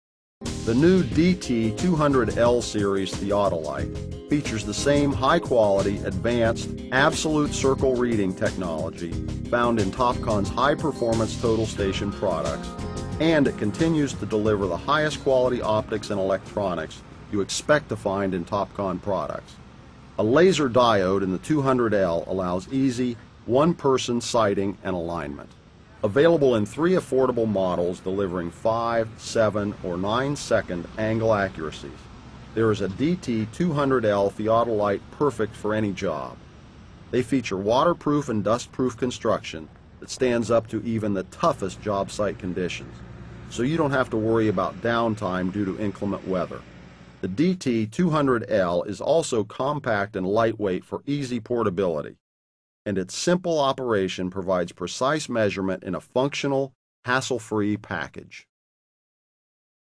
Topcon DT 200L www bouwlasers nl
Met de DT-200 serie introduceert Topcon een serie elektronische theodolieten, waarin de meest recente ontwikkelingen op  zowel optisch- als elektronisch gebied zijn geïntegreerd. De nieuwe DT-200 serie theodolieten hebben nu dezelfde hoge kwaliteit absolute rand afleestechnologie als de Topcon Total Stations en biedt daarmee de hoogste nauwkeurigheid en betrouwbaarheid. De DT-200 serie is volledig waterdicht en kan onder de zwaarste omstandigheden gebruikt worden. De DT-202 en DT-205 zijn voorzien van een compensator voor de verticale as om een meetnauwkeurigheid van resp.  2" (0.6mgon) of 5" (1.5mgon) te garanderen. Het corrigerend bereik van deze compensator bedraagt ± 3'. Met de DT-202/205 en 207 bent u verzekerd van hoge nauwkeurigheid dankzij het tweezijdige afleessysteem. De DT-202 en 205 hebben 1" (0.2mgon) als kleinste afleeswaarde voor zowel de horizontale- als de verticale hoeken. De DT-202 en DT-205 beschikken over een RS-232C seriële poort, die communicatie met een extern veldgeheugen en computers mogelijk maakt.